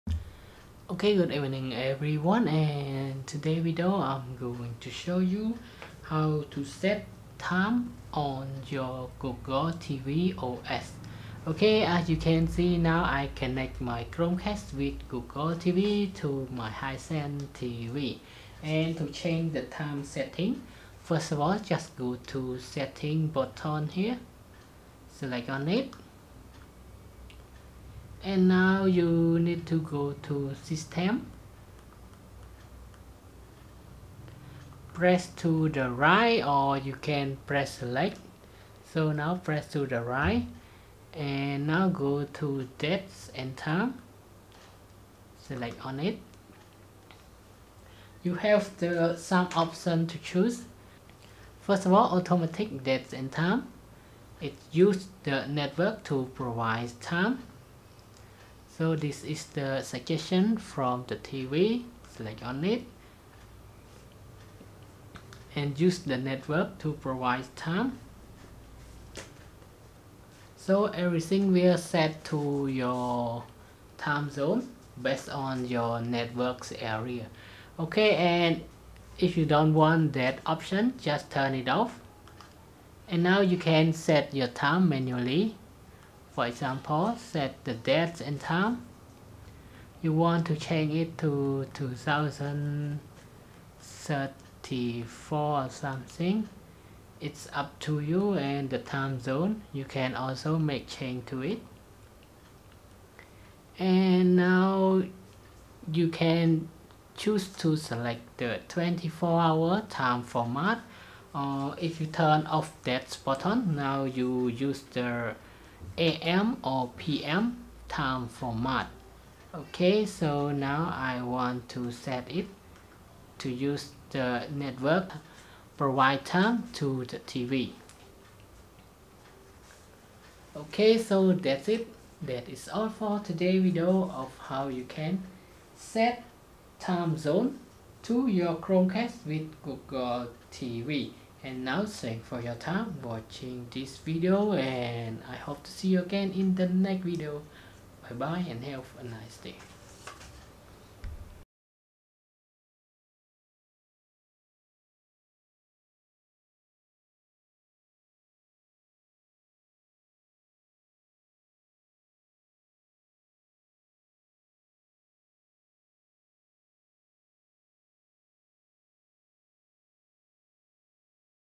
How to Set Date and Time on Chromecast Google TV
In today's video, I am going to show you how to set date and time on Chromecast Google TV.

You can set clock to auto set following your network area. On the other hand, you also can set it manually by your own settings. Let see detail in the video.